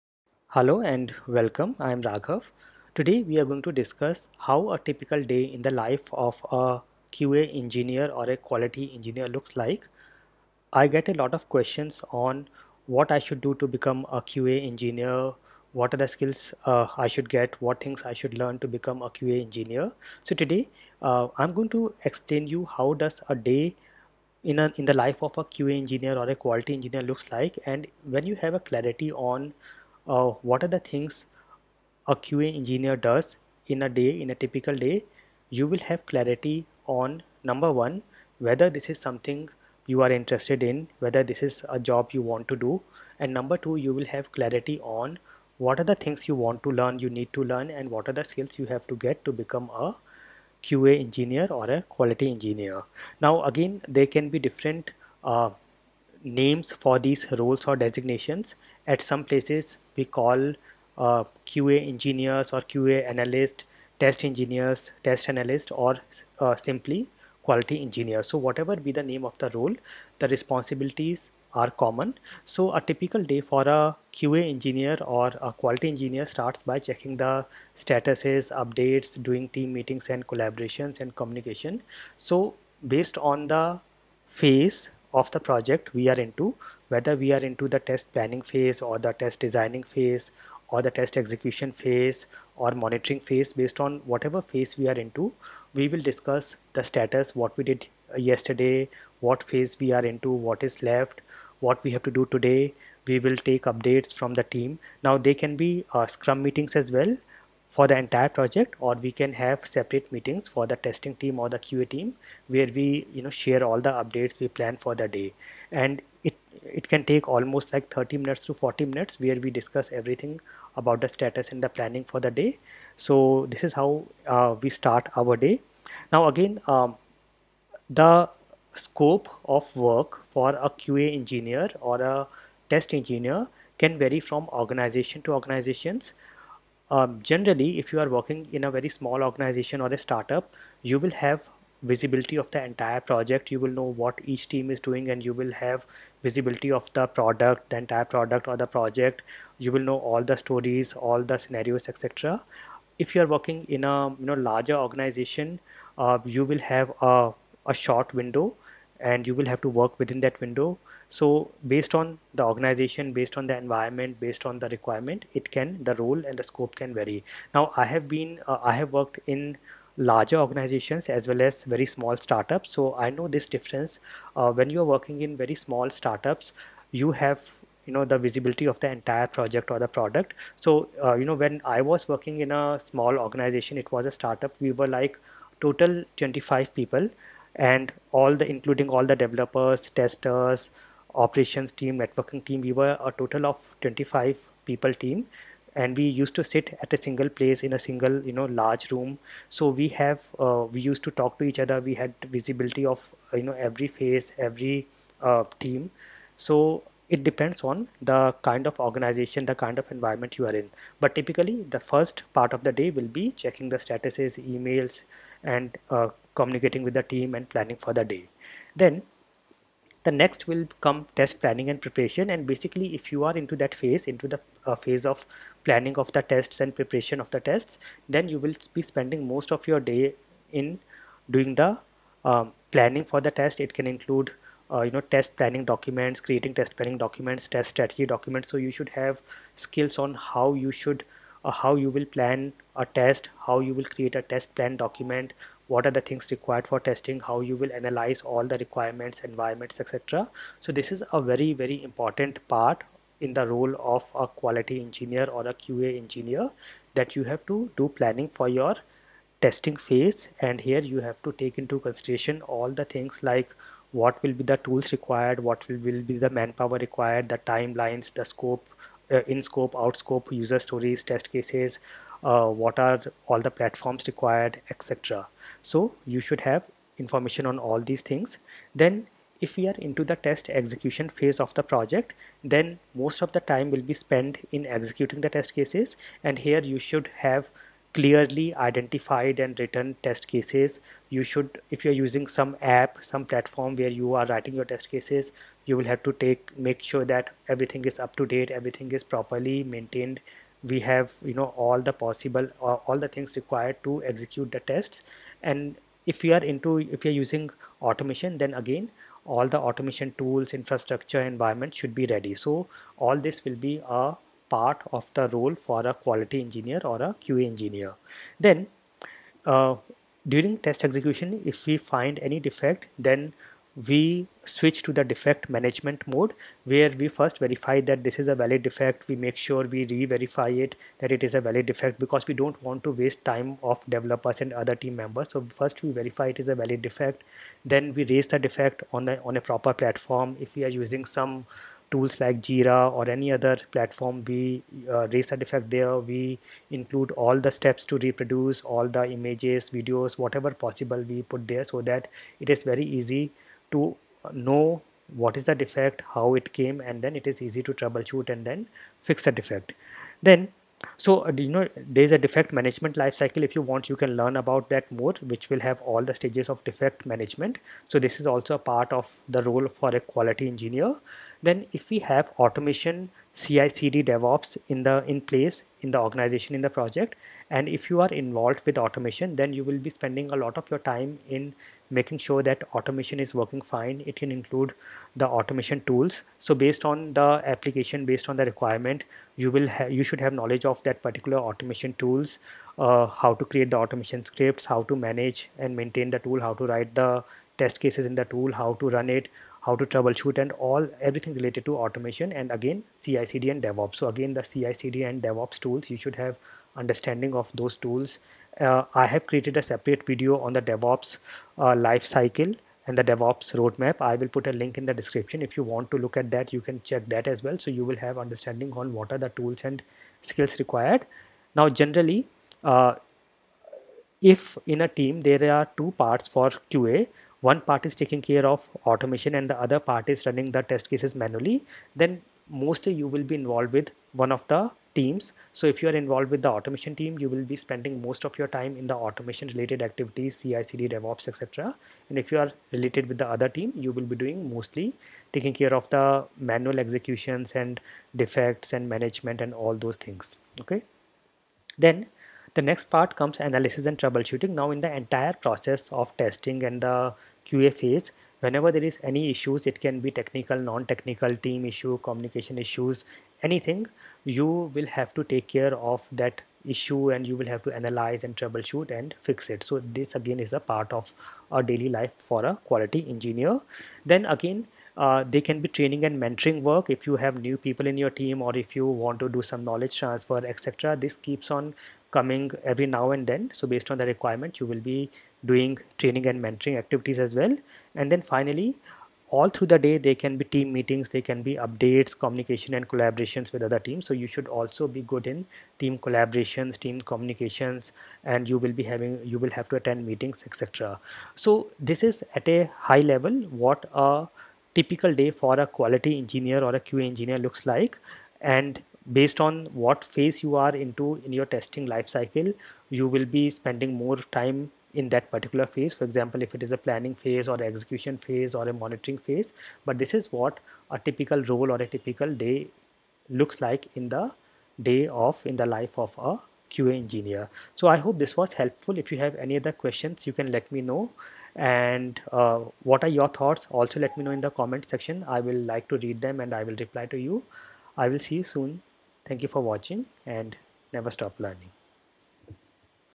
AskRaghav | A day in life of a QA Engineer
What does a QA, QE, Test Engineer or Test Analyst do in a day
00:00  Intro
01:12  Start the day - checking status, emails, communication

03:39  Test planning and preparation

04:39  Test Execution

05:18  Defect management

06:11  Automation | CI CD DevOps

07:32  Analysis & Troubleshooting

07:54  Training & Mentoring

08:07  Team Meetings, Updates, Communication & Collaboration

A day in life of Quality Engineer
What does a QA Engineer do in a typical day

Raghav Pal

▬▬▬▬ USEFUL LINKS ▬▬▬▬